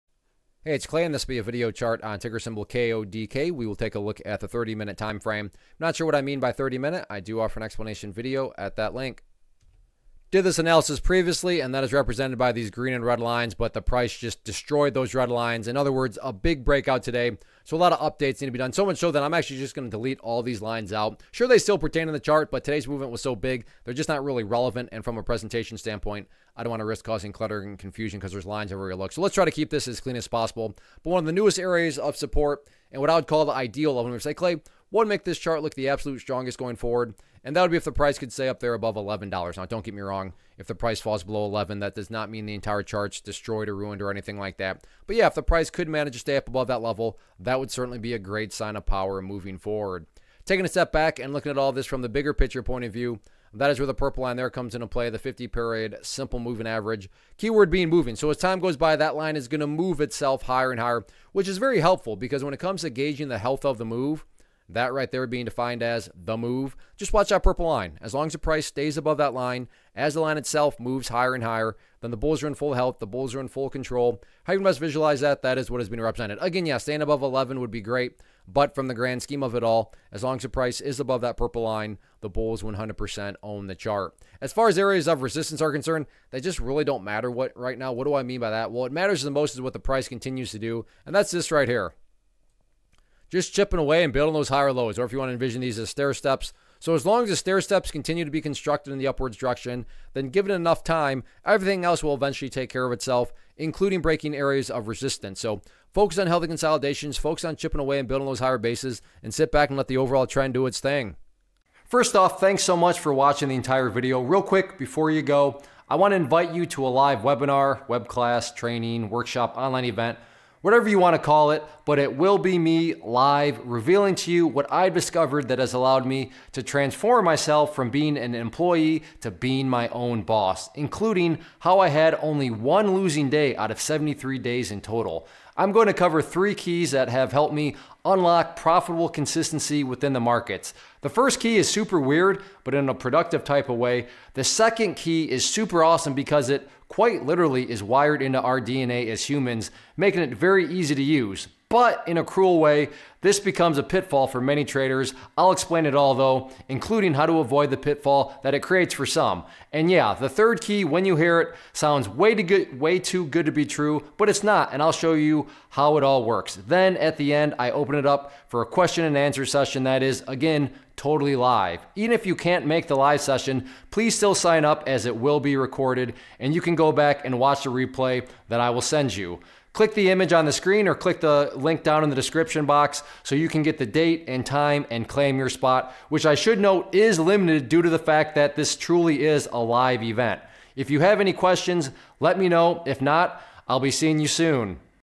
Eastman Kodak Company - KODK Stock Chart Technical Analysis for 09-18-2020
Learn how to read stock charts and identify technical patterns as ClayTrader does a quick stock chart review on Eastman Kodak Company (KODK).